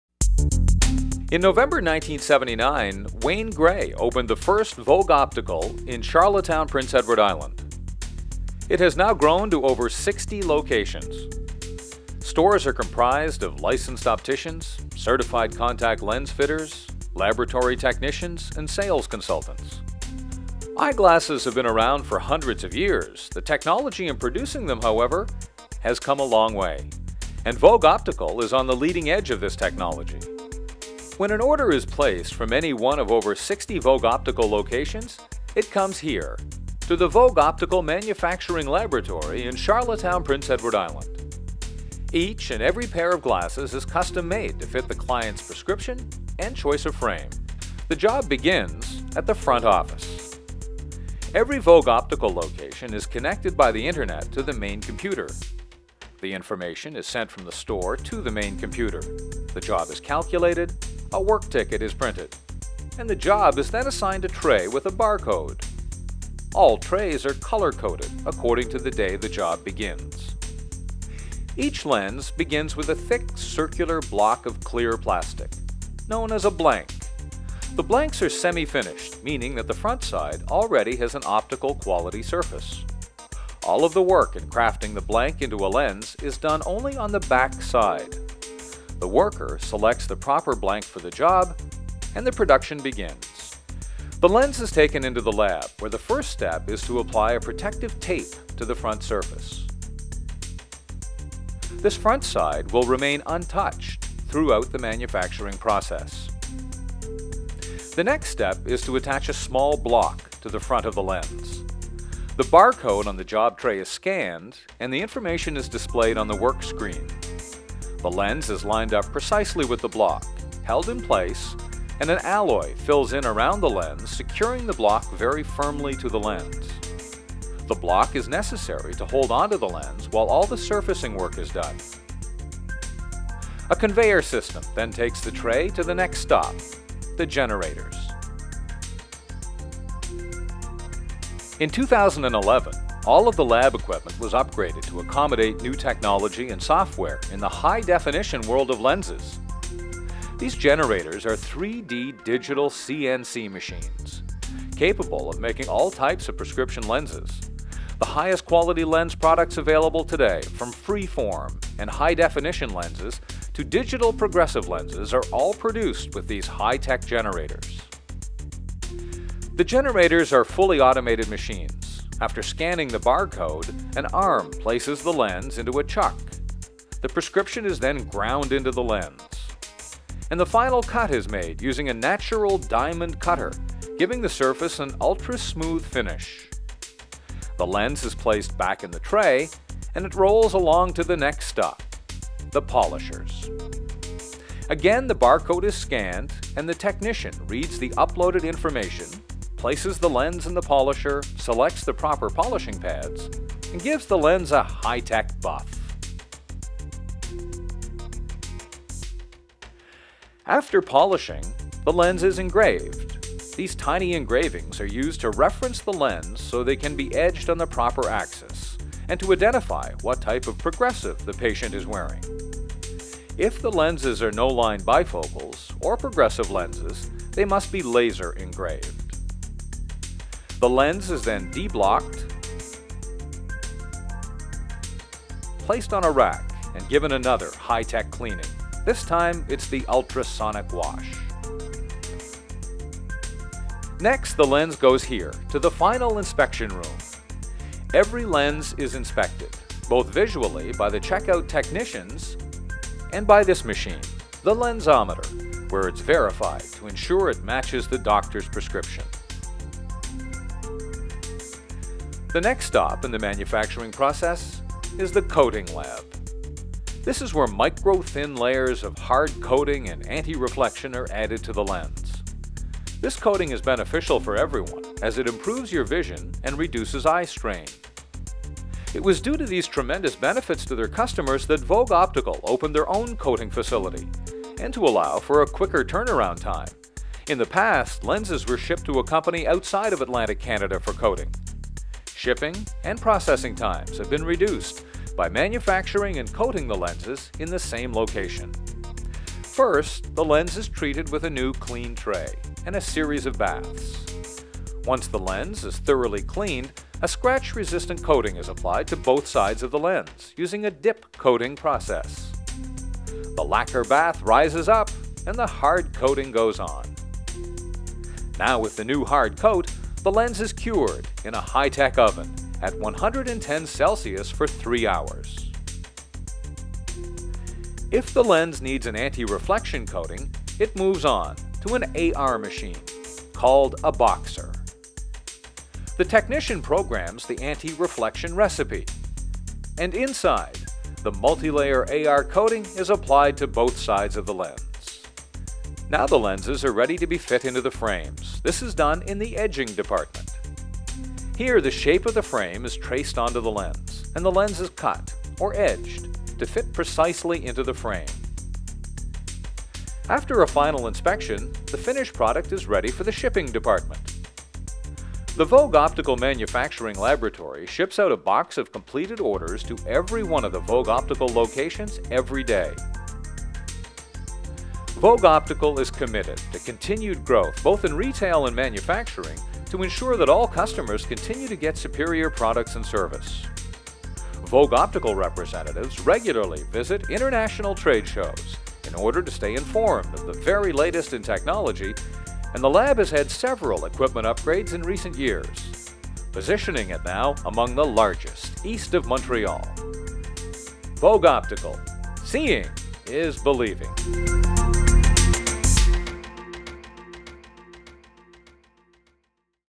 Vogue Optical Manufacturing Laboratory Tour
Ever wonder how glasses are made? This is a video of the Vogue Optical Manufacturing Laboratory in Prince Edward Island.